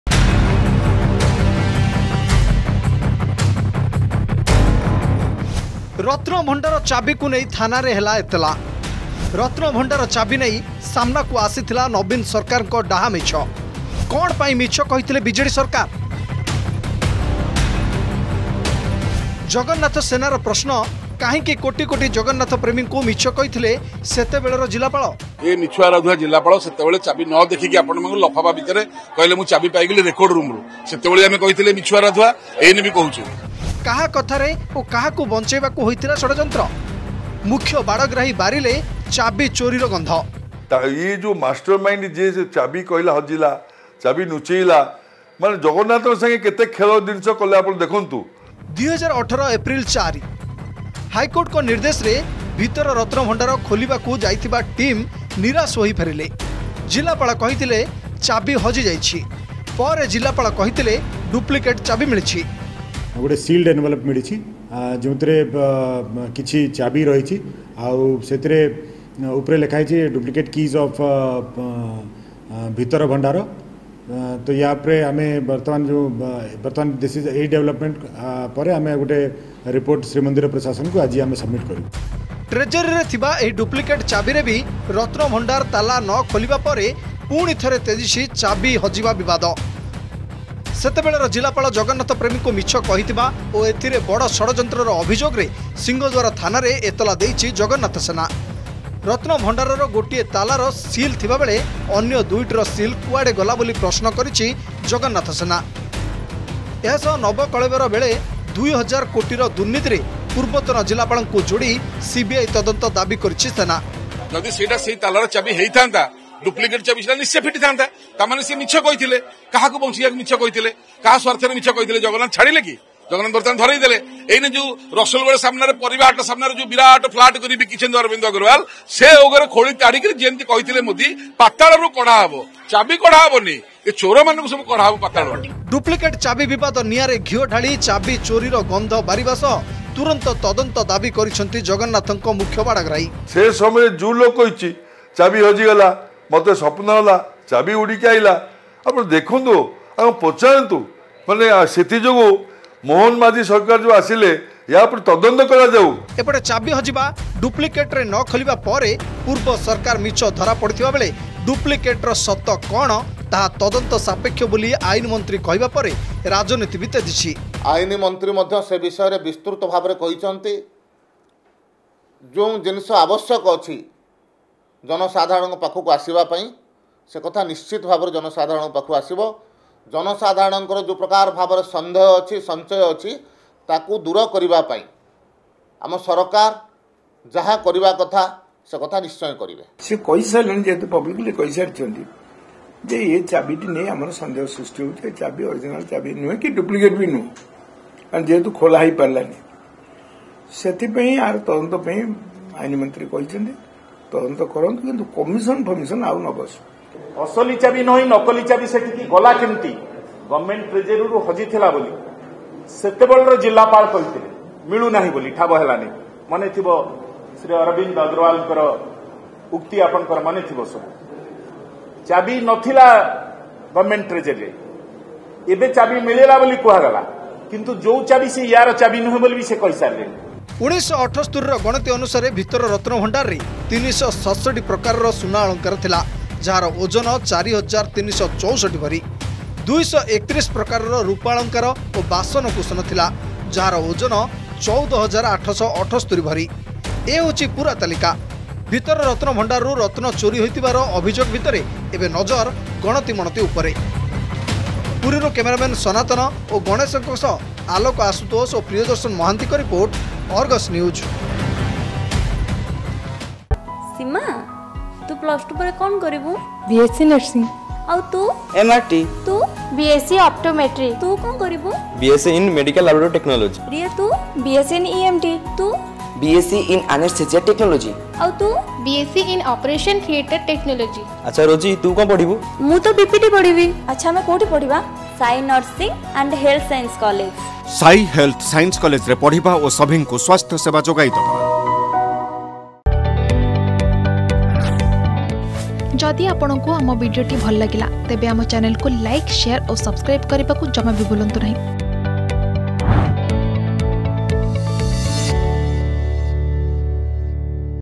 ରତ୍ନ ଭଣ୍ଡାର ଚାବିକୁ ନେଇ ଥାନାରେ ହେଲା ଏତଲା || Ratnabhandar Key
ରତ୍ନ ଭଣ୍ଡାର ଚାବିକୁ ନେଇ ଥାନାରେ ଦିଆଯାଇଛି ଏତଲା । ଜଗନ୍ନାଥ ସେନା ପକ୍ଷରୁ ଏତଲା ଦିଆଯାଇଥିବା ବେଳେ, ଜଗନ୍ନାଥ ସେନା ପ୍ରଶ୍ନ କରିଛି, କାହିଁକି କୋଟି କୋଟି ଜଗନ୍ନାଥ ପ୍ରେମୀଙ୍କୁ ମିଛ କହିଥିଲେ ସେତେବେଳର ଜିଲ୍ଲାପାଳ । ଆଉ ଏସବୁ ଭିତରେ ଜଗନ୍ନାଥଙ୍କ ମୁଖ୍ୟ ବାଡ଼ଗ୍ରାହୀ ବାରିଛନ୍ତି, ଚାବି ଚୋରିର ଗନ୍ଧ ।